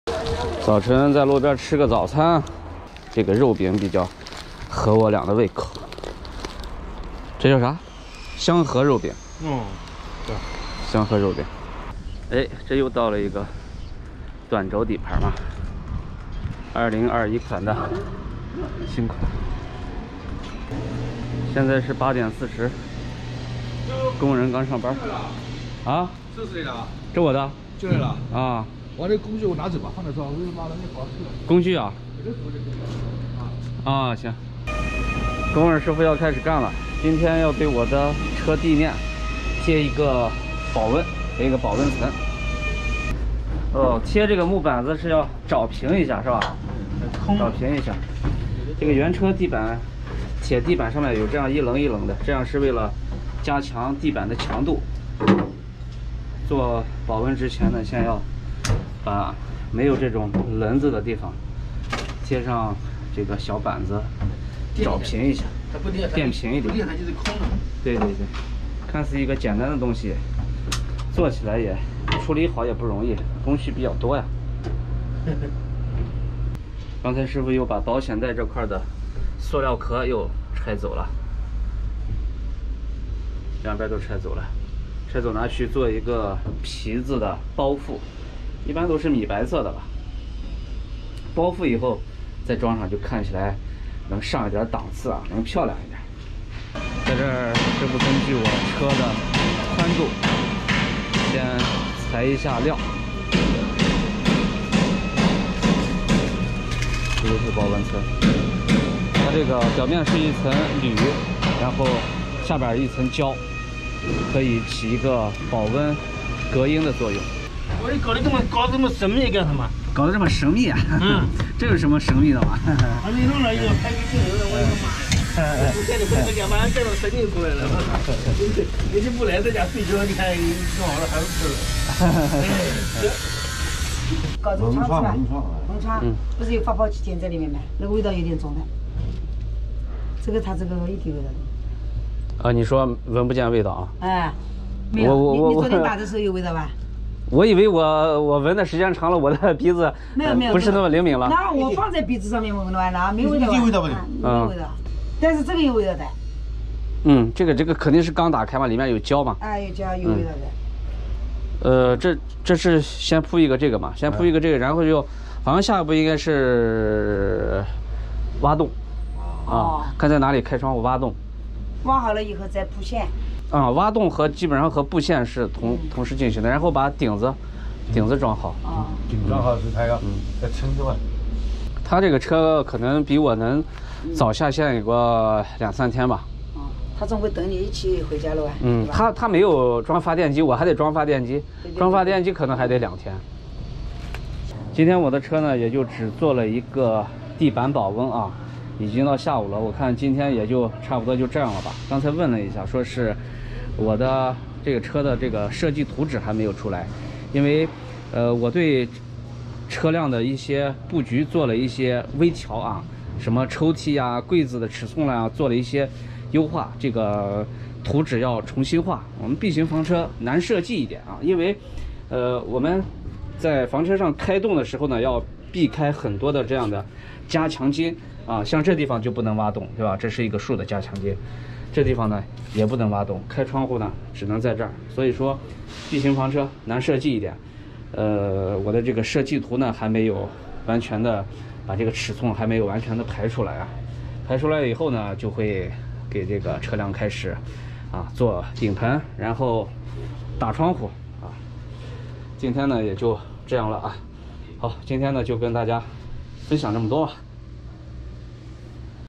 刚订车的大哥大嫂，来参观浩东的房车，听他们如何评价发泡保温？
刚订车的大哥大嫂，来参观浩东的房车，听他们如何评价发泡保温？！~浩東房車遊記
大家好，我是浩東，這是我的youtube唯壹官方頻道。